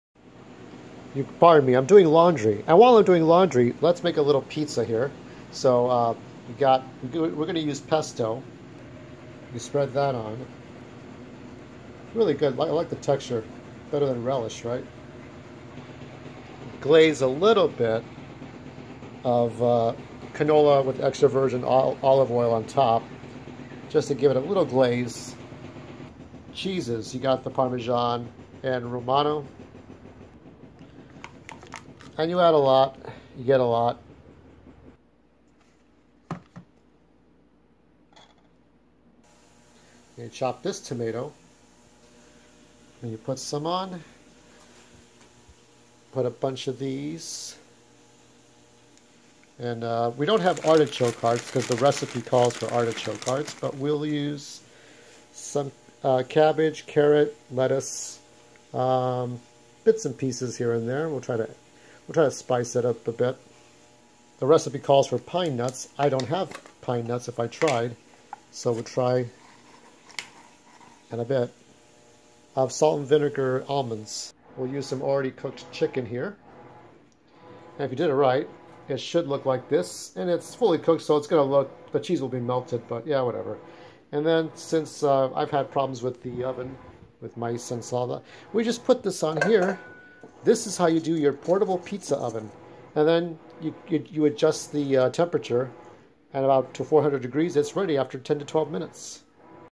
Food 4 Post: The portable pizza cook. 🍕
A recipe a followed from Market Pantry foods for Chicken Pesto pizza.